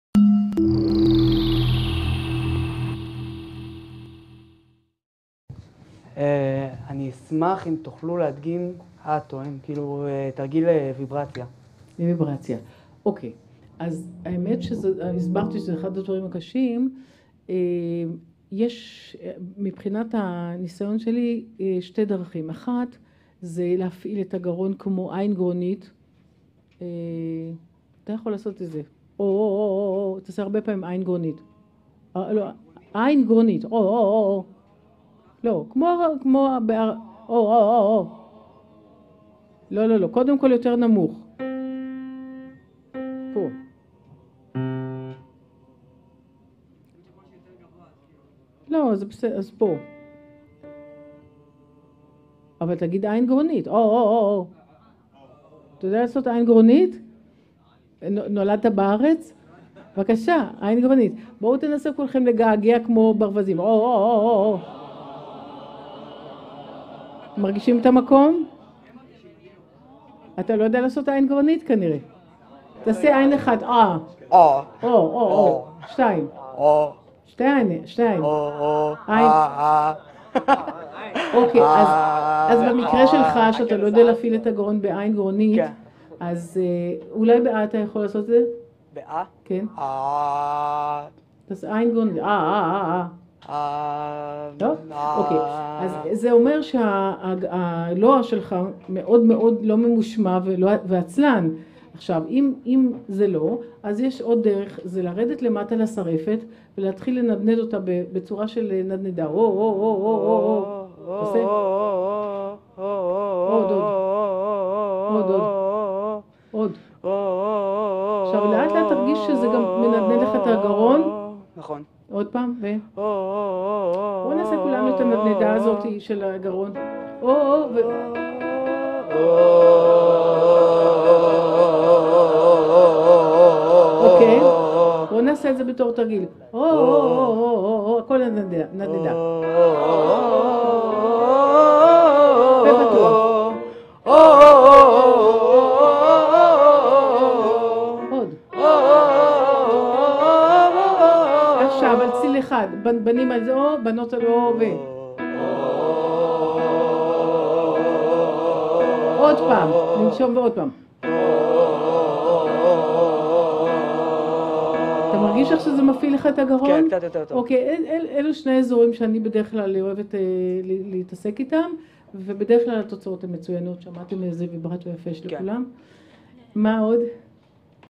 הדגמת תרגיל ויברציה עם רחל הוכמן
תשובתה של רחל הוכמן, ראש המחלקה לפיתוח קול בבית הספר למוסיקה של הקריה האקדמית אונו

רחל, אשר מלמדת בטכניקת "נטורה קנטו" (שירה טבעית) ומתמקדת בסגנונות המוסיקה הקלה-פופ, רוק ומחזות זמר, משתפת מניסיונה העשיר חוויות שנצברו במהלך השנים, תוך כדי הדגמה מעשית של התרגילים על תלמידיה (זמרים מקצועיים מן השורה הראשונה)